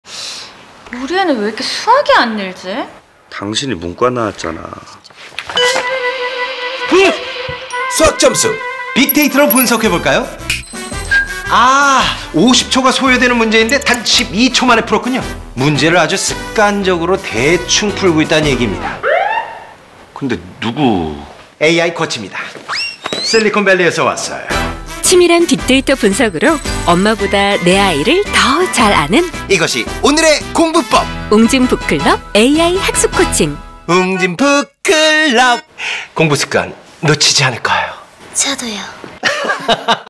[웅진북클럽] 엄마보다 내 아이를 더 잘아는 웅진북클럽 AI학습코칭
우리 애는 왜 수학 성적이 안 오를까?
책상에 앉아는 있는데…

학습 습관을 바로 잡아야 성적이 오른다!

엄마도 몰랐던 우리 아이 학습 습관의 발견
웅진북클럽 AI학습코칭

▶특별한 혜택! 자세히 알아볼까요?